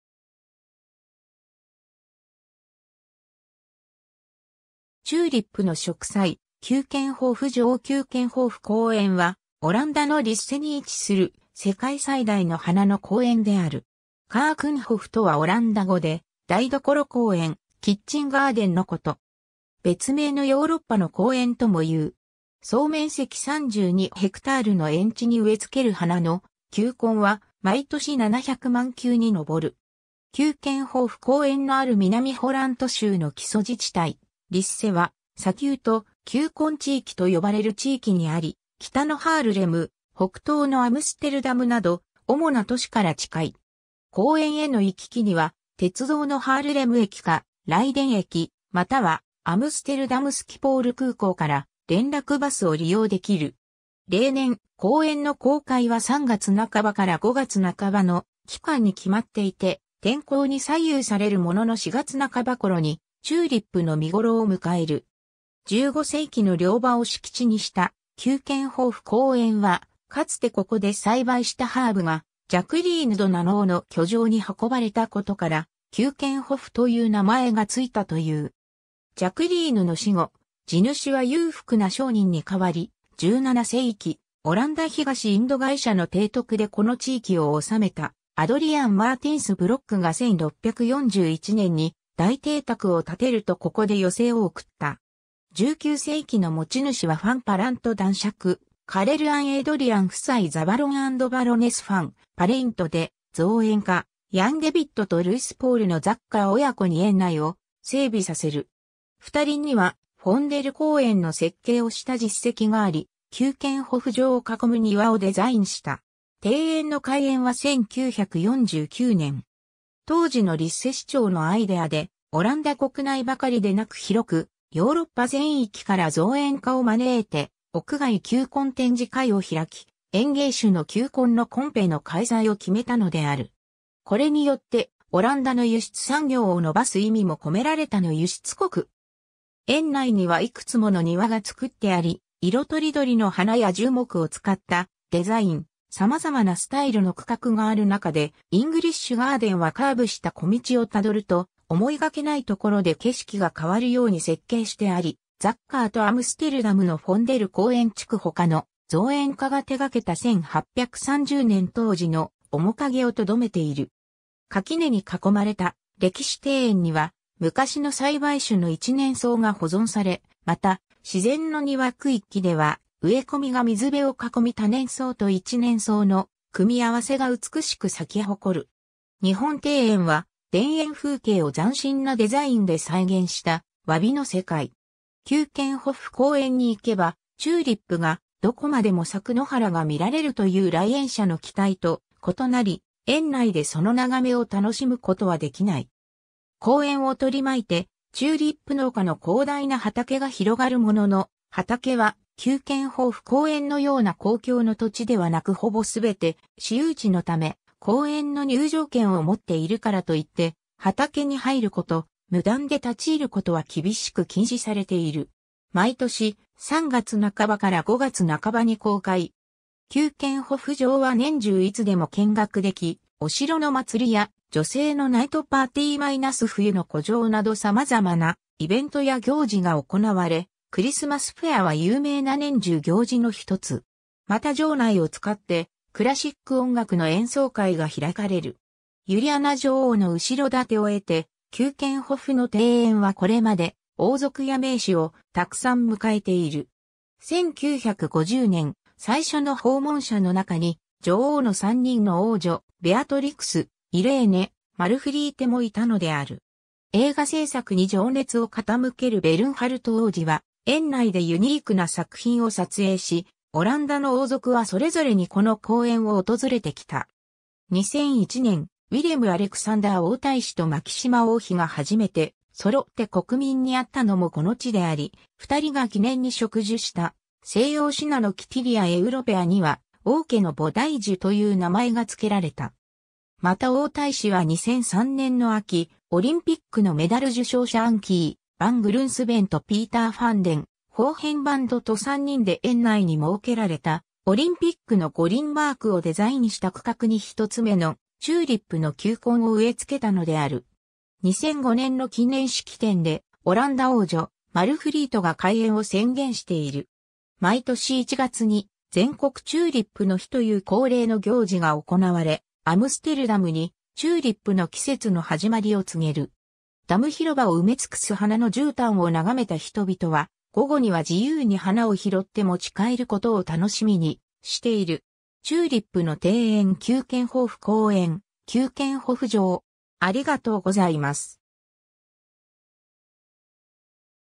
キューケンホフ公園
チューリップの植栽 (2009年) キューケンホフ城 キューケンホフ公園（キューケンホフこうえん、Keukenhof）は、オランダのリッセに位置する世界最大の花の公園である。
Keukenhof（蘭: [ˈkøːkə(n)ˌɦɔf]）とはオランダ語で「台所公園」「キッチンガーデン」のこと。
別名のヨーロッパの公園ともいう。
 キューケンホフ公園のある南ホラント州の基礎自治体リッセは「砂丘と球根地域」と呼ばれる地域にあり、北のハールレム、北東のアムステルダムなど主な都市から近い。
公園への行き来には鉄道のハールレム駅かライデン駅、またはアムステルダム・スキポール空港から連絡バスを利用できる。
 例年、公園の公開は3月半ばから5月半ばの期間に決まっていて、天候に左右されるものの4月半ばころにチューリップの見ごろを迎える。
 15世紀の猟場を敷地にしたキューケンホフ公園は、かつてここで栽培したハーブがジャクリーヌ・ド・ナノーの居城に運ばれたことから「キューケンホフ」 (城の台所に花を届けた敷地) という名前がついたという。
ジャクリーヌの死後、地主は裕福な商人に代わり、17世紀、オランダ東インド会社の提督でこの地域を治めたAdriaen Maertensz Blockが1641年に大邸宅 (現在のキューケンホフ城) を建てるとここで余生を送った。
19世紀の持ち主はファン・パラント男爵カレルアン=エイドリアン夫妻 the Baron and Baroness Van Pallandt で、造園家 Jan DavidとLouis PaulのZocher親子に園内を整備させる。
ふたりにはフォンデル公園の設計をした実績があり、キューケンホフ城を囲む庭をデザインした。
 庭園の開園は1949年。
当時のリッセ市長のアイデアで、オランダ国内ばかりでなく広くヨーロッパ全域から造園家を招いて屋外球根展示会を開き、園芸種の球根のコンペの開催を決めたのである。
これによってオランダの輸出産業を伸ばす意味もこめられた (オランダは世界最大の花卉 (かき) の輸出国)。
 園内にはいくつもの庭が作ってあり、色とりどりの花や樹木を使ったデザイン、さまざまなスタイルの区画があるなかでイングリッシュ・ガーデンはカーブした小道をたどると思いがけないところで景色が変わるように設計してあり、Zocher とアムステルダムのフォンデル公園地区ほかの造園家が手がけた1830年当時のおもかげをとどめている。
垣根に囲まれた歴史庭園には昔の栽培種の一年草が保存され、また自然の庭区域では植え込みが水辺を囲み多年草と一年草の組み合わせが美しく咲き誇る。
日本庭園は田園風景を斬新なデザインで再現した、わびの世界。
キューケンホフ公園に行けばチューリップがどこまでも咲く野原が見られるという来園者の期待と異なり、園内でその眺めを楽しむことはできない。
公園を取り巻いてチューリップ農家の広大な畑が広がるものの、畑はキューケンホフ公園のような公共の土地ではなくほぼすべて私有地のため、公園の入場券を持っているからと言って畑に入ること、無断で立ち入ることは厳しく禁止されている。
 毎年、3月半ばから5月半ばに公開 (2015年は3月20日から5月17日、花のパレードは5月25日) 。
 キューケンホフ城は年中いつでも見学でき、お城の祭りや「女性のナイトパーティー - 冬の古城」などさまざまなイベントや行事が行われ、クリスマスフェアは有名な年中行事のひとつ 。
また城内を使ってクラシック音楽の演奏会が開かれる。
 ユリアナ女王の後ろ盾を得て、キューケンホフの庭園はこれまで王族や名士をたくさん迎えている。
1950年、最初の訪問者のなかに女王の3人の王女ベアトリクス、イレーネ、マルフリーテもいたのである。
映画制作に情熱を傾けるベルンハルト王子は園内でユニークな作品を撮影し、オランダの王族はそれぞれにこの公園を訪れてきた。
2001年、ウィレム・アレクサンダー王太子 (当時) とマキシマ王妃が初めて揃って国民に会ったのもこの地であり、ふたりが記念に植樹したセイヨウシナノキ Tilia europea には「王家の菩提樹」("Koningslinde") という名前がつけられた。
また王太子は2003年の秋、オリンピックのメダル受賞者アンキー・ヴァン・グルンスヴェン (馬術) ...